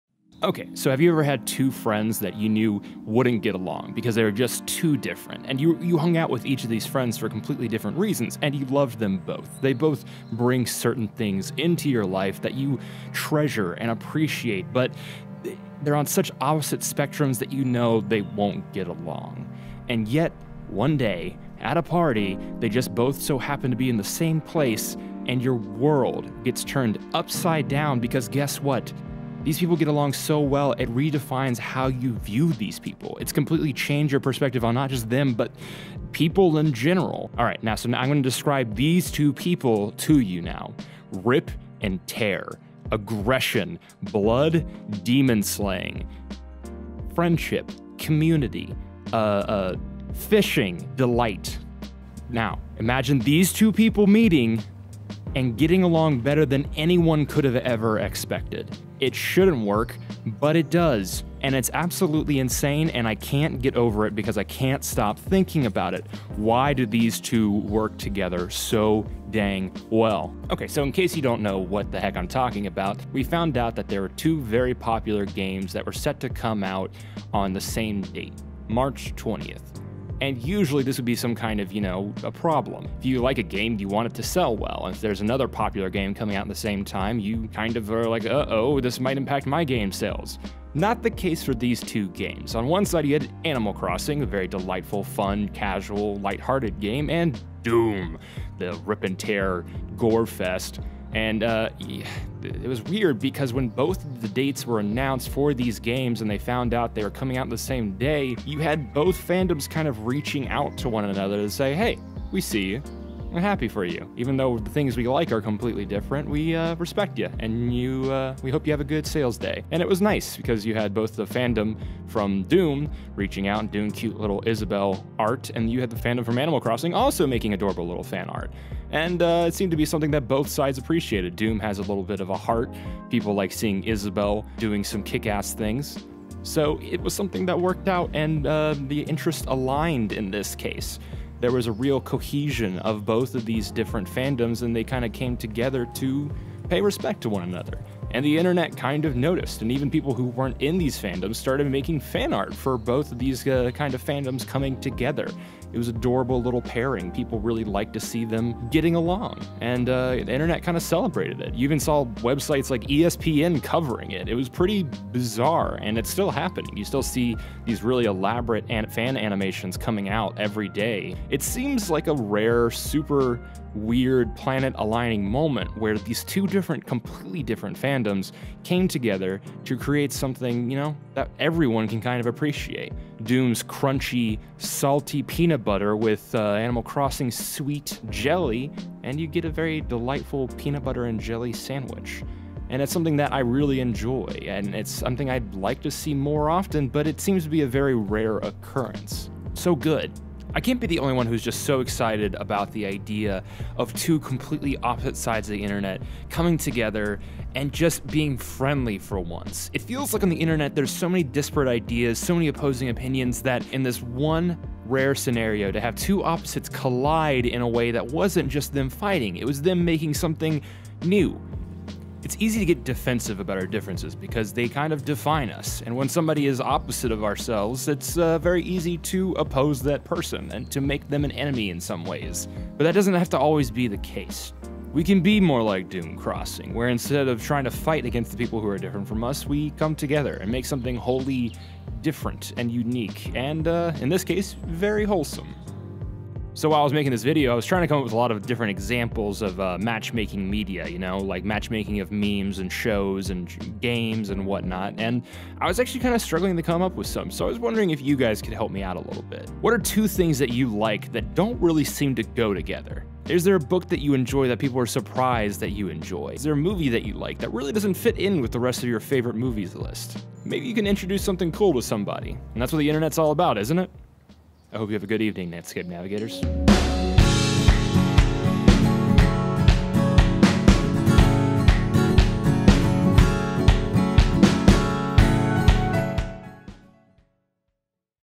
Doomguy x Isabelle: Can you ship fandoms?
We've seen Isabelle and Doom Guy paired up countless times at this point but why don't we see more meme's paired up like this? Why is it that the Animal Crossing fandom and the Doom: Eternal Fandom get along but not other fandoms? Could other memes come together to create something even greater?

The art in the thumbnail was done by rariatto(ganguri)! I did not get their permission to use it! My bad! Love your work!